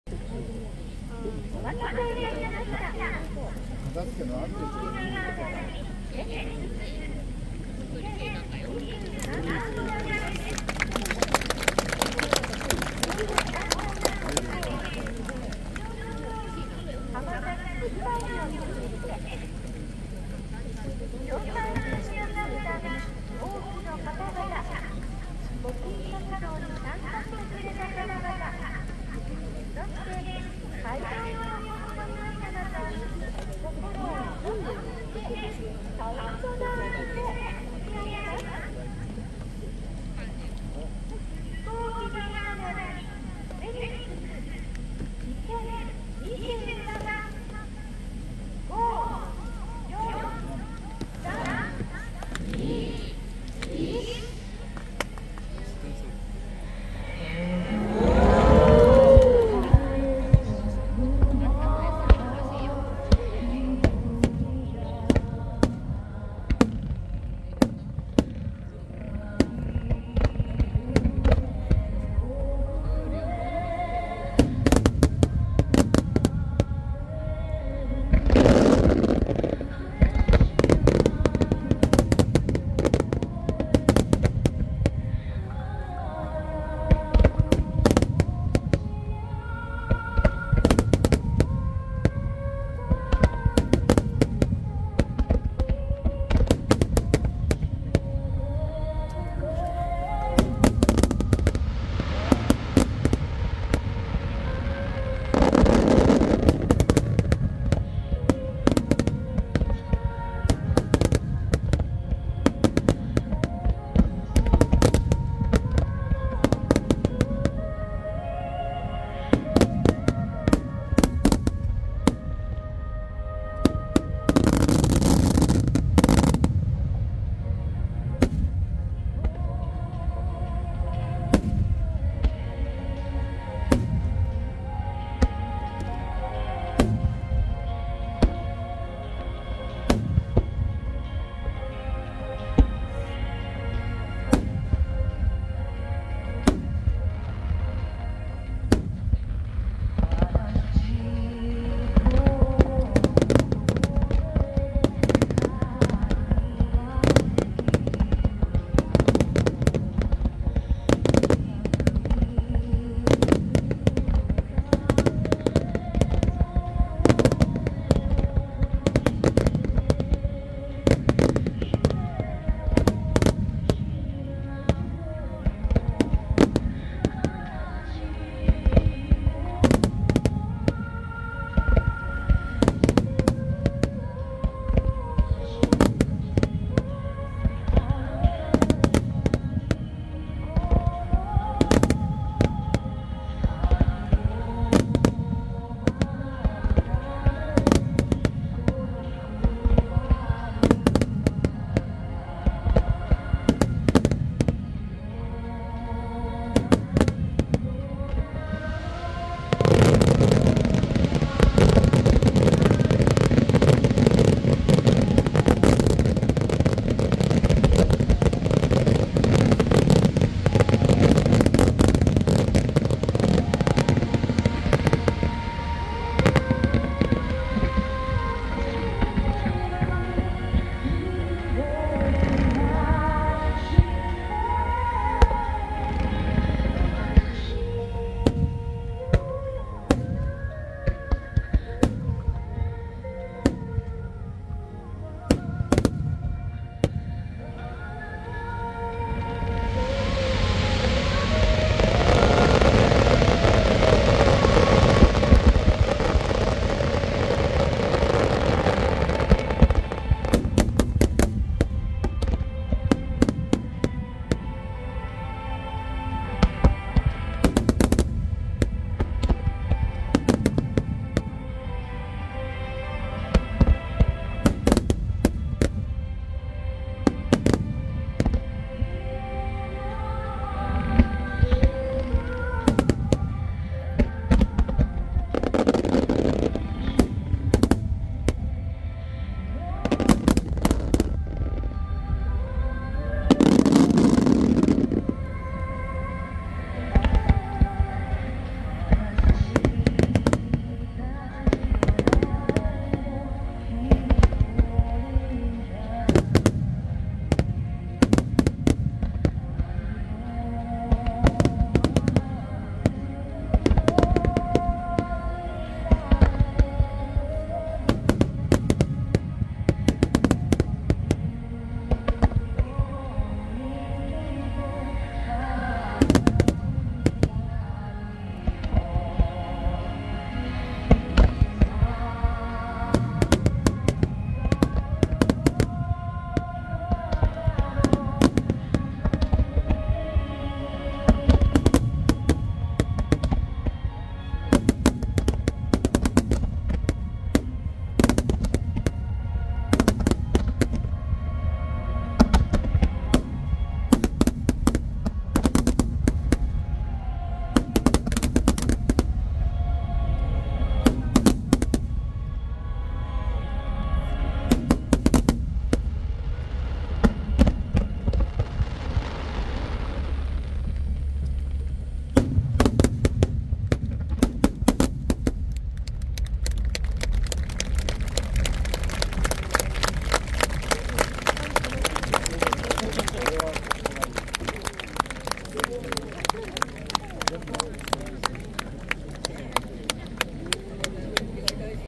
長岡まつり大花火大会2023/8/2 右岸フェニックステーブル席より
俯瞰ではなく現場映像です。
Galaxy S23 Ultraにて撮影。